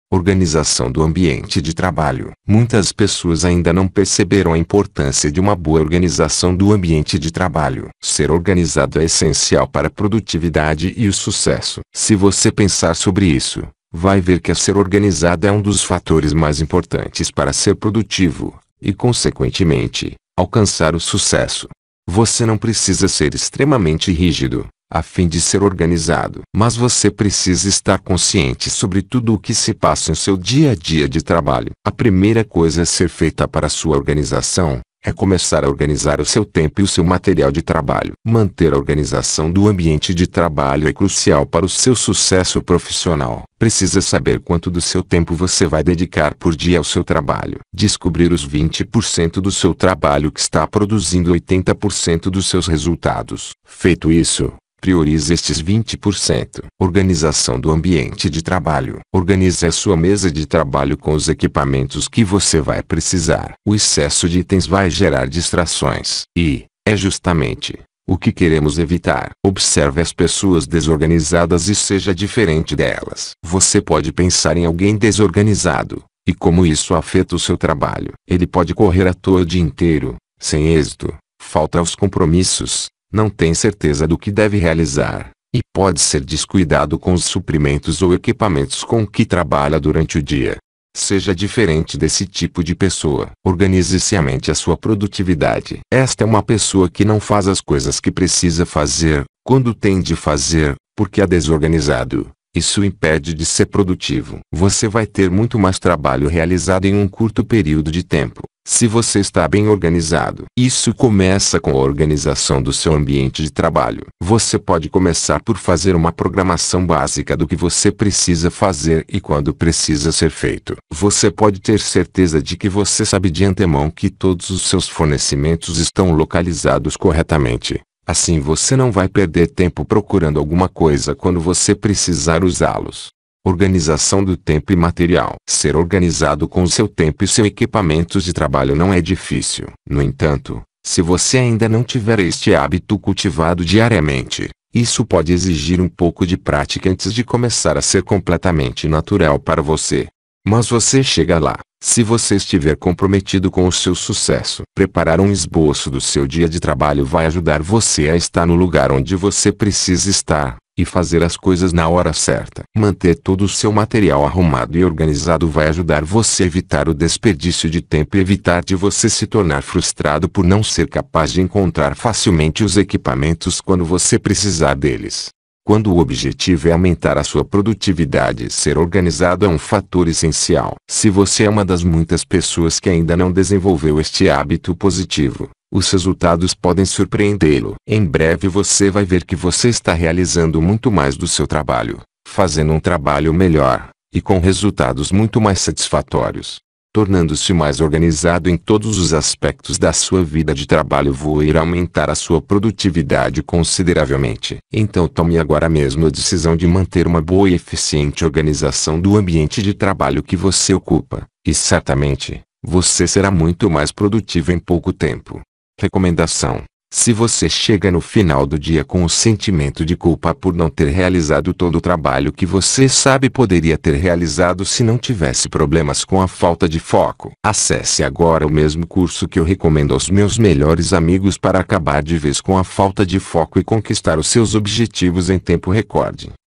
Organização do ambiente de trabalho
Muitas pessoas ainda não perceberam a importância de uma boa organização do ambiente de trabalho. Ser organizado é essencial para a produtividade e o sucesso.

Se você pensar sobre isso, vai ver que ser organizado é um dos fatores mais importantes para ser produtivo, e consequentemente, alcançar o sucesso.

Você não precisa ser extremamente rígido, a fim de ser organizado. Mas você precisa estar consciente sobre tudo o que se passa em seu dia a dia de trabalho.

A primeira coisa a ser feita para sua organização, é começar a organizar o seu tempo e o seu material de trabalho. Manter a organização do ambiente de trabalho é crucial para o seu sucesso profissional.

Precisa saber quanto do seu tempo você vai dedicar por dia ao seu trabalho. Descobrir os vinte por cento do seu trabalho que está produzindo 80 por cento dos seus resultados. Feito isso, priorize estes vinte por cento.

Organize a sua mesa de trabalho com os equipamentos que você vai precisar. O excesso de itens vai gerar distrações. E, é justamente, o que queremos evitar.

Observe as pessoas desorganizadas e seja diferente delas.

Você pode pensar em alguém desorganizado, e como isso afeta o seu trabalho. Ele pode correr à toa o dia inteiro, sem êxito, falta aos compromissos, não tem certeza do que deve realizar, e pode ser descuidado com os suprimentos ou equipamentos com que trabalha durante o dia.

Seja diferente desse tipo de pessoa. Organize-se e aumente a sua produtividade.

Esta é uma pessoa que não faz as coisas que precisa fazer, quando tem de fazer, porque é desorganizado, isso o impede de ser produtivo.

Você vai ter muito mais trabalho realizado em um curto período de tempo, se você está bem organizado. Isso começa com a organização do seu ambiente de trabalho.

Você pode começar por fazer uma programação básica do que você precisa fazer e quando precisa ser feito.

Você pode ter certeza de que você sabe de antemão que todos os seus fornecimentos estão localizados corretamente, assim você não vai perder tempo procurando alguma coisa quando você precisar usá-los.

Organização do tempo e material

Ser organizado com o seu tempo e seu equipamentos de trabalho não é difícil. No entanto, se você ainda não tiver este hábito cultivado diariamente, isso pode exigir um pouco de prática antes de começar a ser completamente natural para você.

Mas você chega lá, se você estiver comprometido com o seu sucesso.

Preparar um esboço do seu dia de trabalho vai ajudar você a está no lugar onde você precisa estar, e fazer as coisas na hora certa.

Manter todo o seu material arrumado e organizado vai ajudar você a evitar o desperdício de tempo e evitar de você se tornar frustrado por não ser capaz de encontrar facilmente os equipamentos quando você precisar deles.

Quando o objetivo é aumentar a sua produtividade ser organizado é um fator essencial.

Se você é uma das muitas pessoas que ainda não desenvolveu este hábito positivo, os resultados podem surpreendê-lo.

Em breve você vai ver que você está realizando muito mais do seu trabalho, fazendo um trabalho melhor, e com resultados muito mais satisfatórios.

Tornando-se mais organizado em todos os aspectos da sua vida de trabalho voe irá aumentar a sua produtividade consideravelmente.

Então tome agora mesmo a decisão de manter uma boa e eficiente organização do ambiente de trabalho que você ocupa, e certamente, você será muito mais produtivo em pouco tempo.